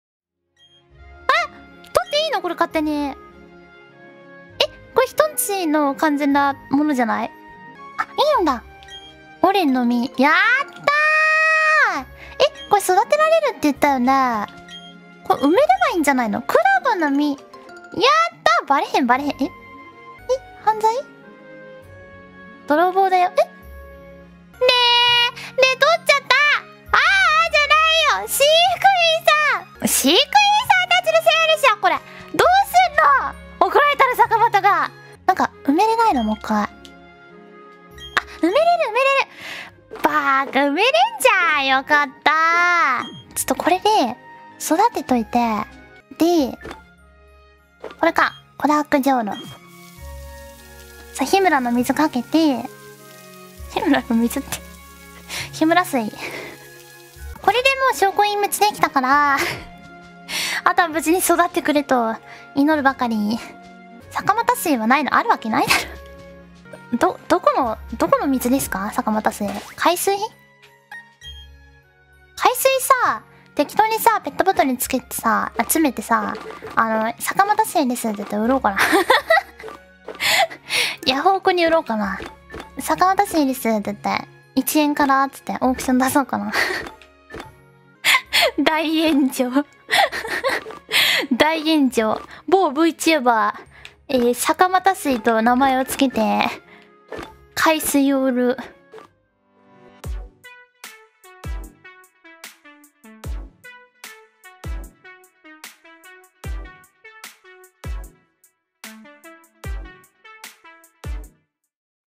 【鹹鹹der~】沙花叉水開賣啦！一元開始競標！？ｗｗｗ【沙花叉クロヱ】
✩影片禁止任何轉載✩
✩翻譯有誤請留言告知✩